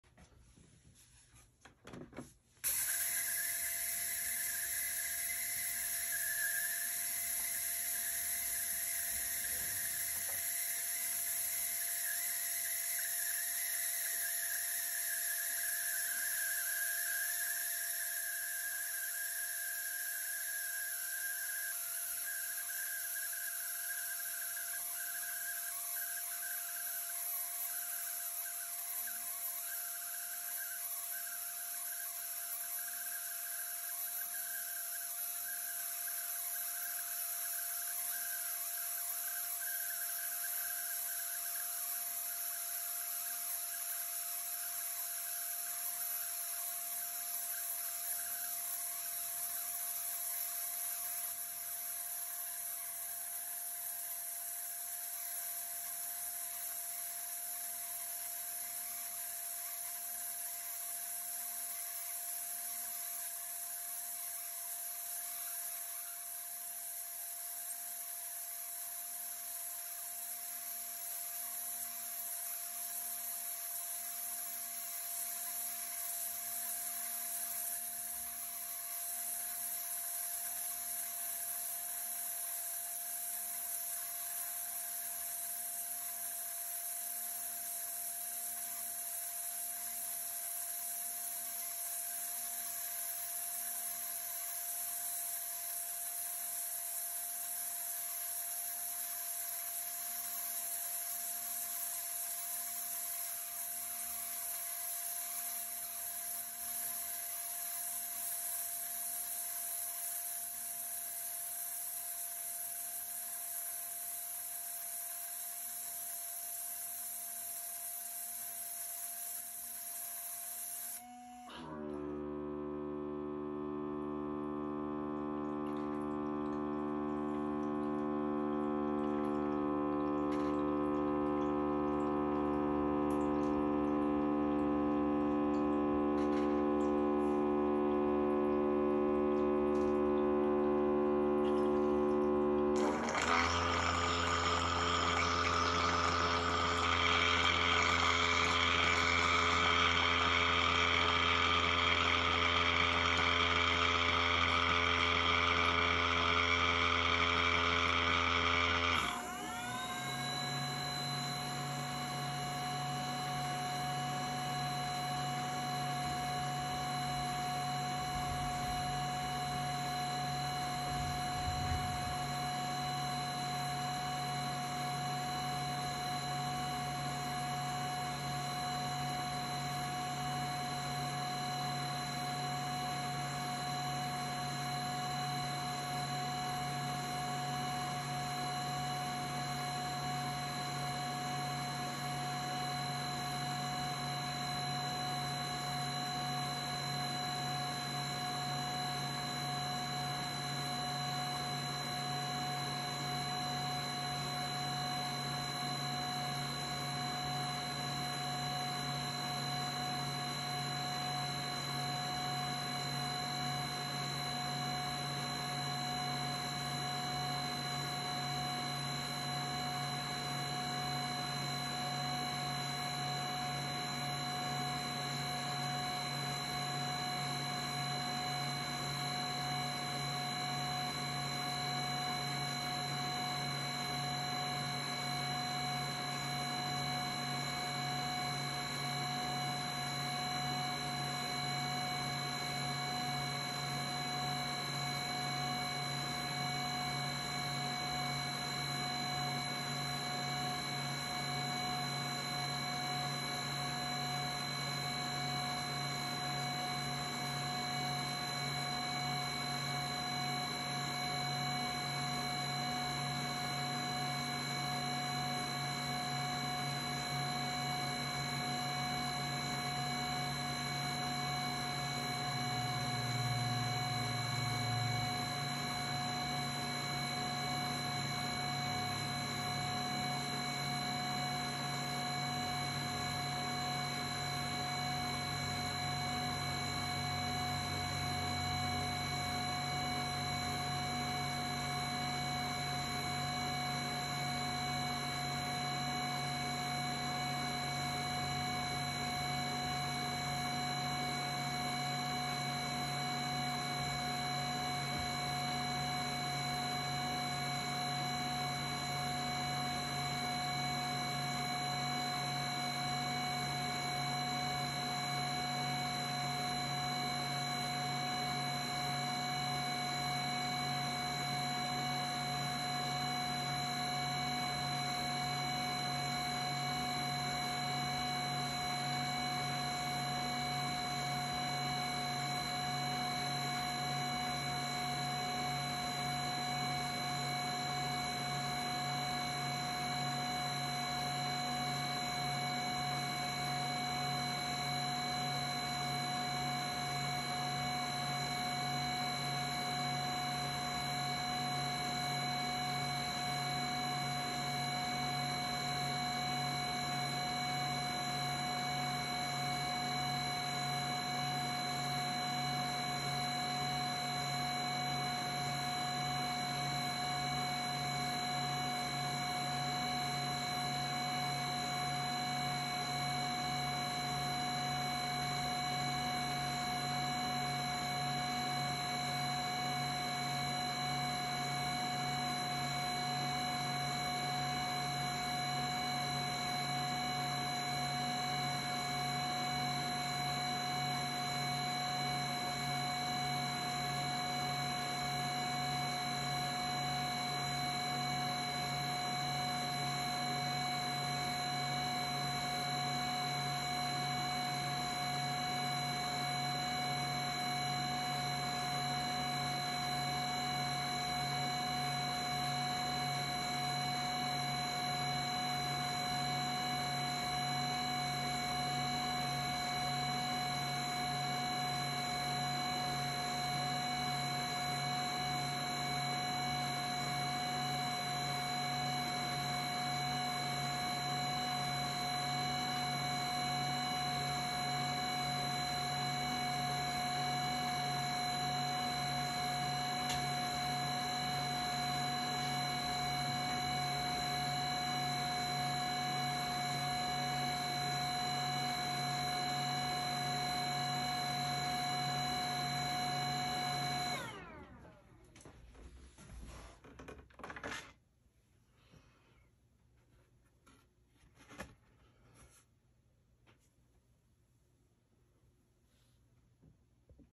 HumminGuru 2/5 cycle
This is what the HumminGuru sounds like for a 2 minute clean followed by a 5 minute drying! It can get annoying if you sit close to it 😁